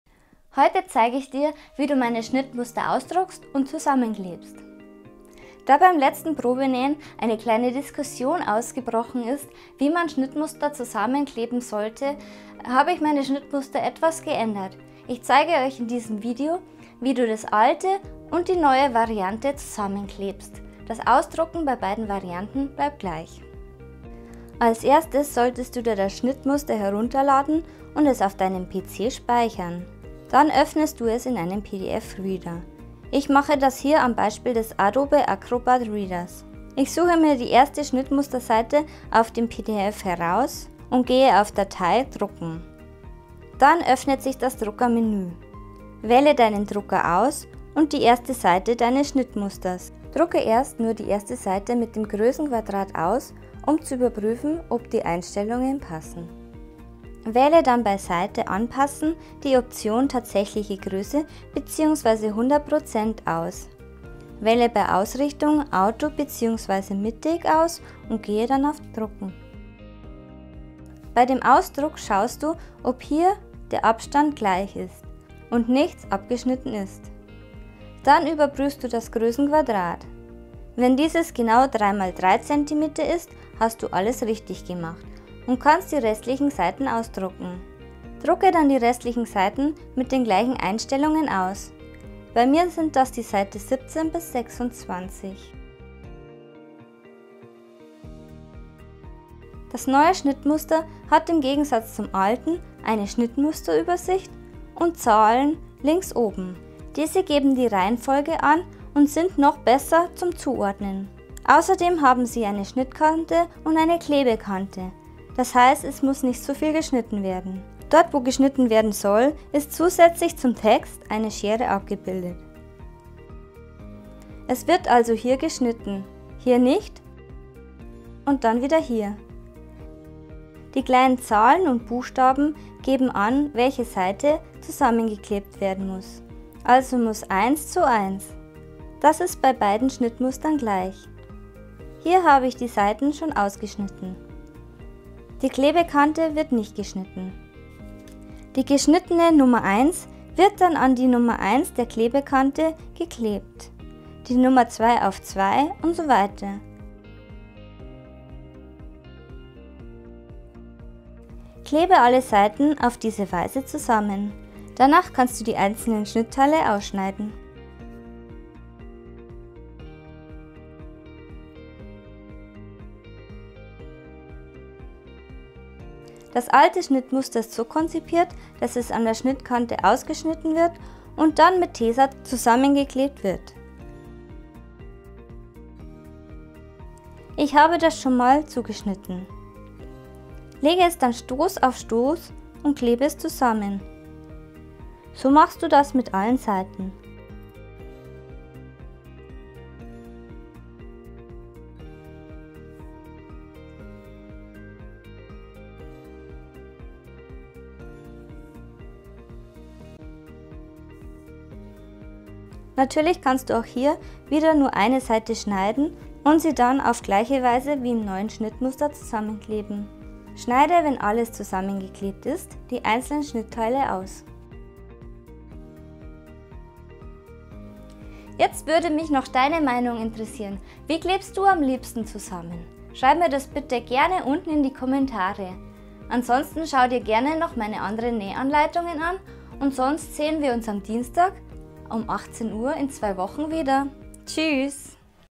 Schnittmuster ausdrucken und zusammenkleben
Heute zeige ich dir, wie du meine Schnittmuster ausdruckst und zusammenklebst.

Viel Spaß beim nachbasteln und nähen.
Eure Jaqueline